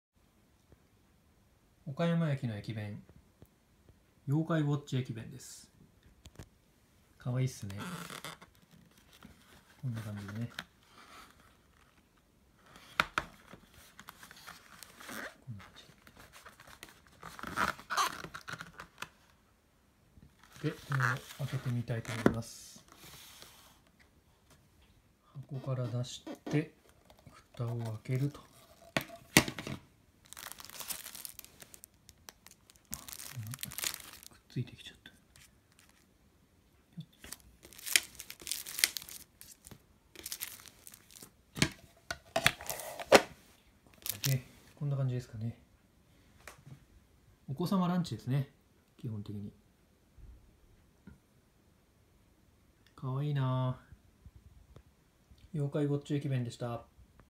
可愛い！妖怪ウォッチ駅弁（岡山駅の駅弁）を開封してみた
岡山駅の駅弁である、「妖怪ウォッチ駅弁」を開封してみたところです。東京駅で購入する事ができました。とても可愛いです。